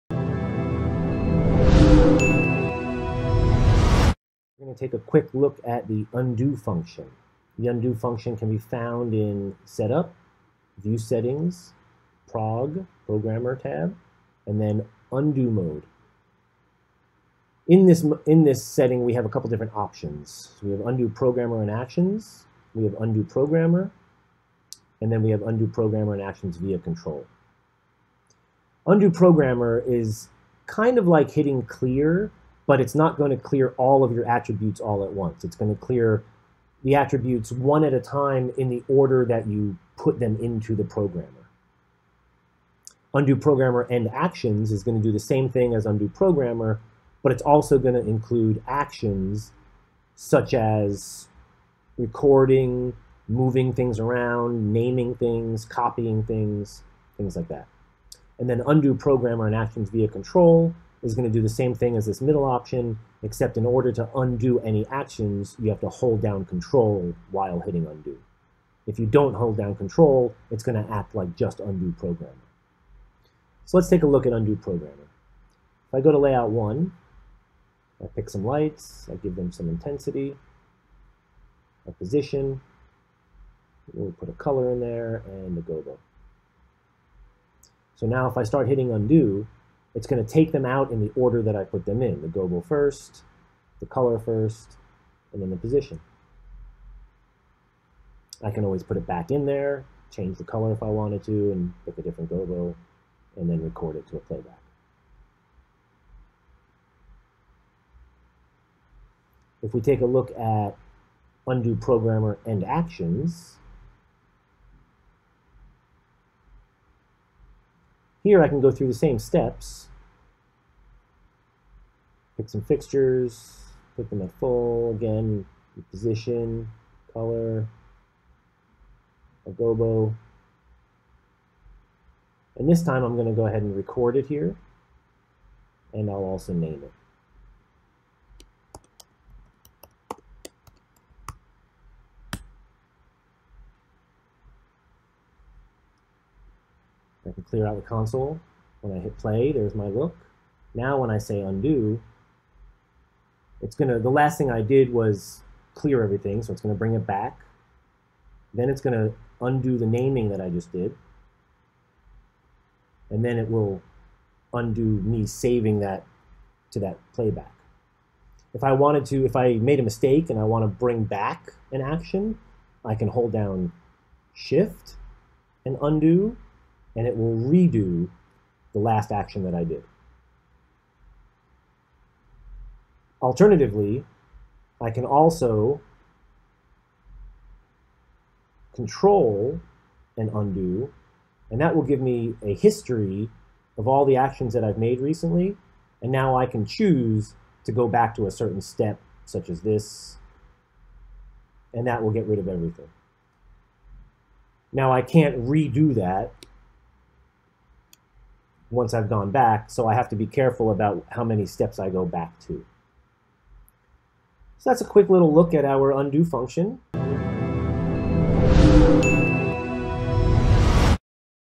MagicQ Feature Vidoes: Undo
MagicQ features an enhanced undo allowing both programmer and actions on the console to be undone.